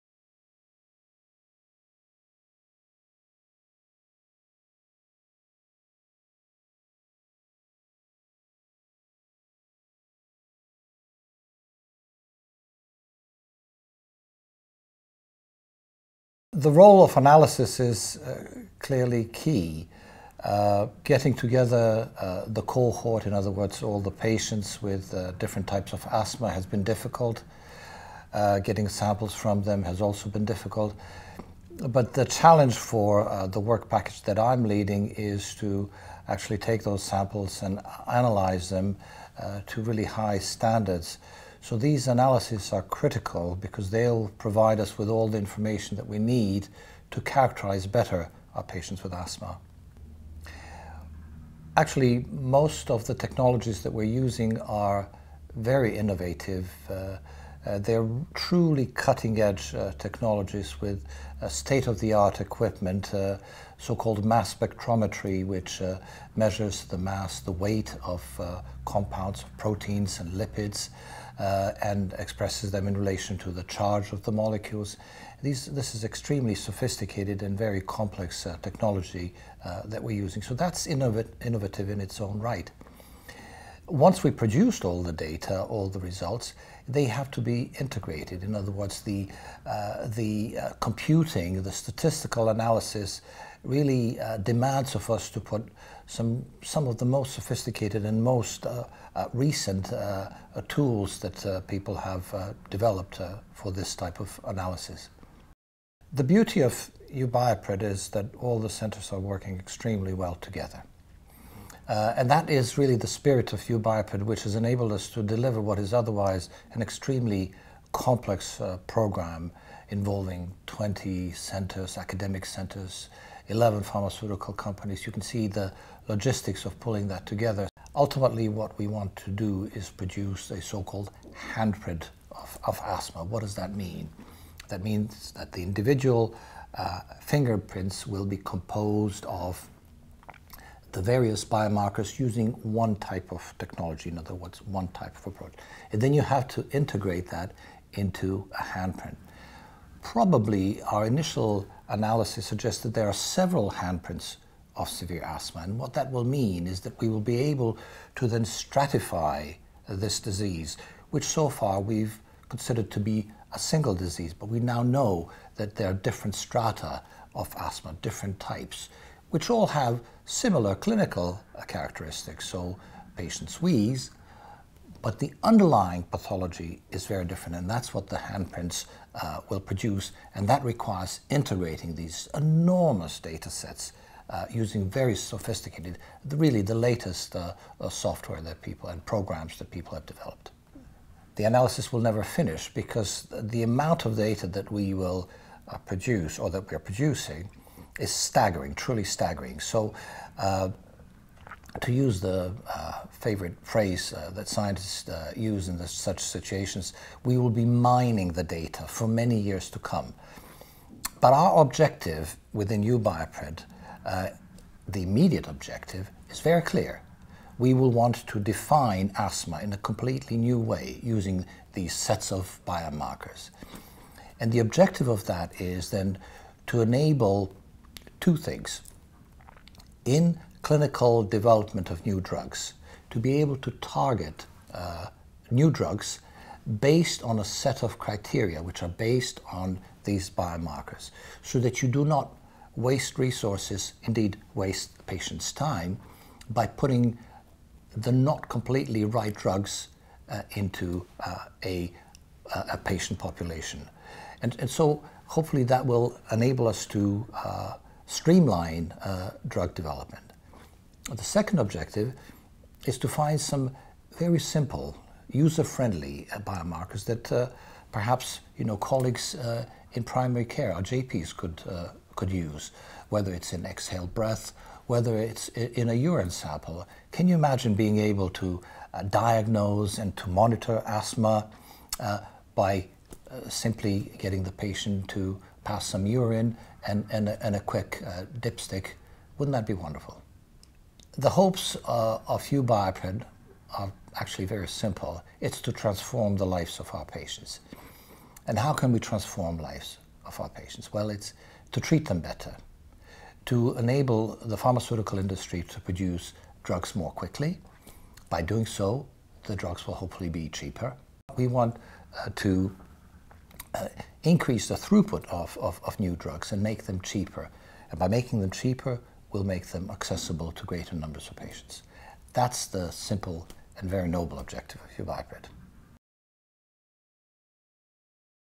Sample analysis in research projects
Professor Ratko Djukanovic describes the role of analysis in a large-scale research project, focusing on the EU-funded U-BIOPRED project.

U-BIOPRED is an IMI project supported by the EU and EFPIA.